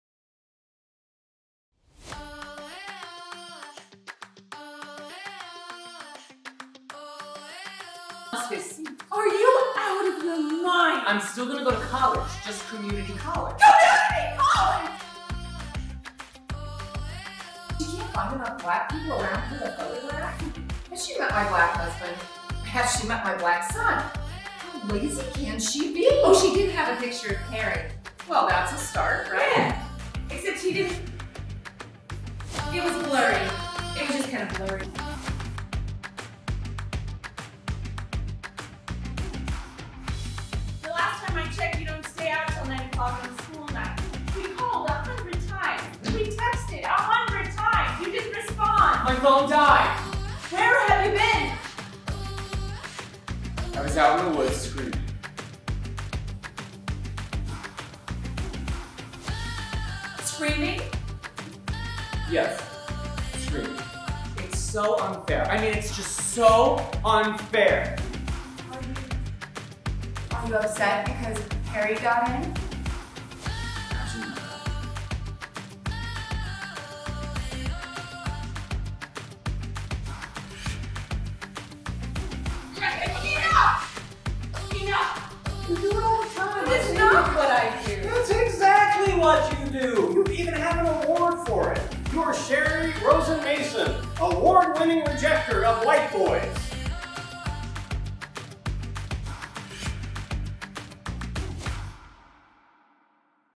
'Admissions' Trailer - Jan 14 - 30, 2022 at the IndyFringe Theatre
We see theatre as a town hall - a place you go to with members of your own community to sort out the issues affecting you all. We seek plays that are urgent, entertaining, timely and spark conversation. 'Admissions' by Joshua Harmon is that play. We hope you can join us! The conversation starts this Friday.